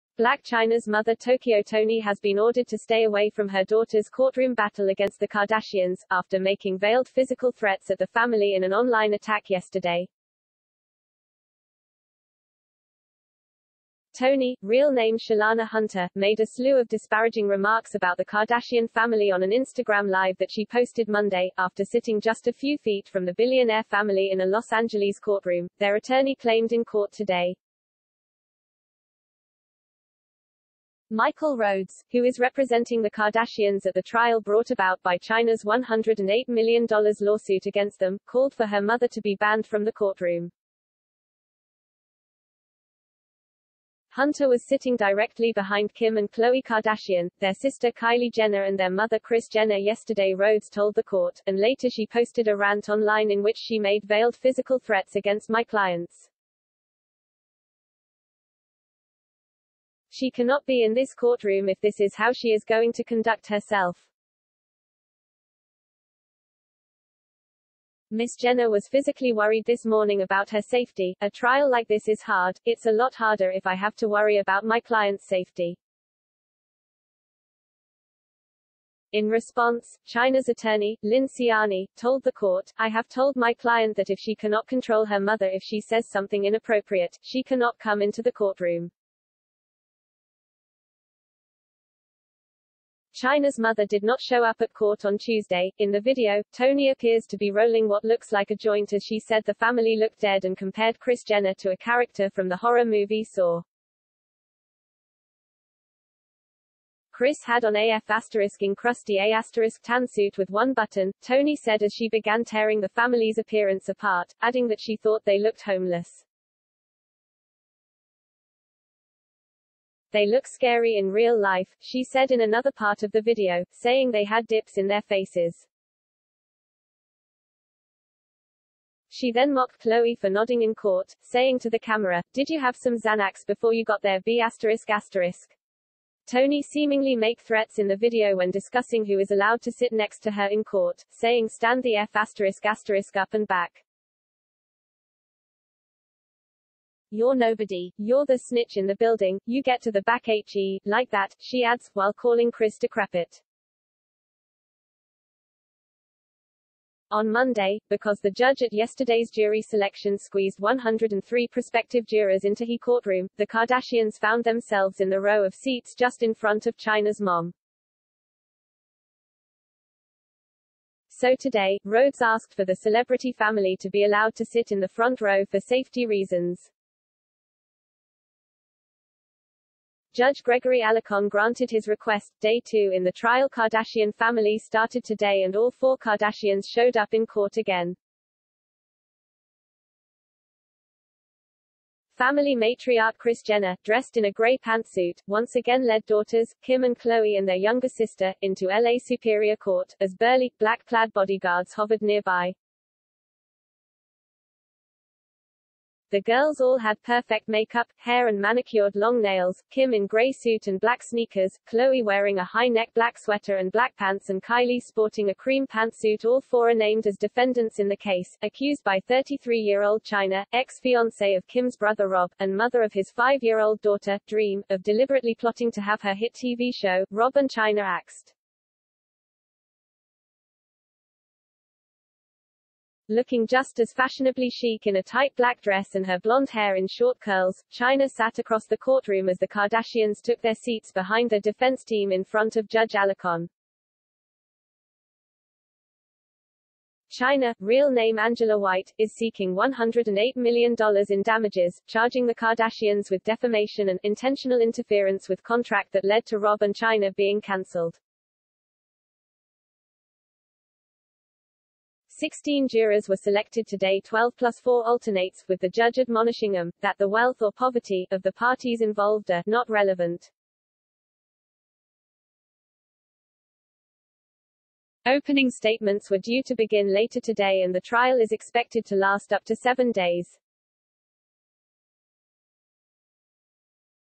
🌍Blac Chyna's mother Tokyo Toni asked to LEAVE courtroom for posting foul-mouthed attack on
Blac Chyna's mother Tokyo Toni has been ordered to stay away from her daughter's courtroom battle against the Kardashians, after making 'veiled physical threats' at the family in an online attack yesterday.

Toni - real name Shalana Hunter - made a slew of disparaging remarks about the Kardashian family on an Instagram Live that she posted Monday, after sitting just a few feet from the billionaire family in a Los Angeles courtroom, their attorney claimed in court today.